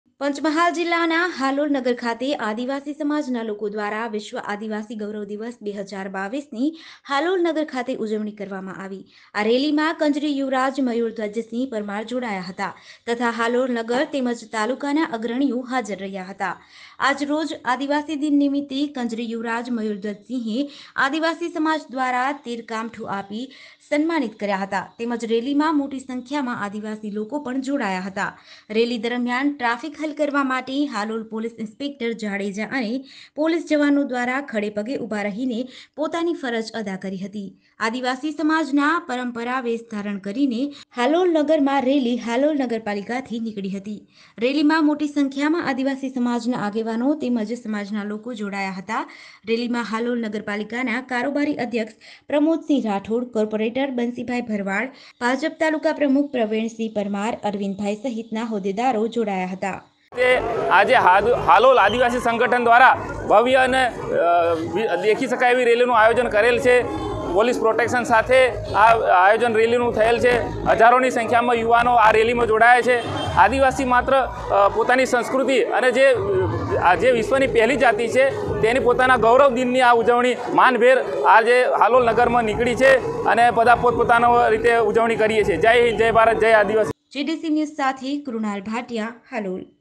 હાલોલ નગર ખાતે આદિવાસી સમાજ દ્વારા 9 ઓગસ્ટ વિશ્વ આદિવાસી દિવસની ઉજવણી,,,
હાલોલ નગર ખાતે આદિવાસી સમાજ દ્વારા 9 ઓગસ્ટ વિશ્વ આદિવાસી દિવસની ઉજવણી કરવામાં આવી જેમાં મોટી સંખ્યામાં આદિવાસી સમાજના લોકો જોડાયા હતા.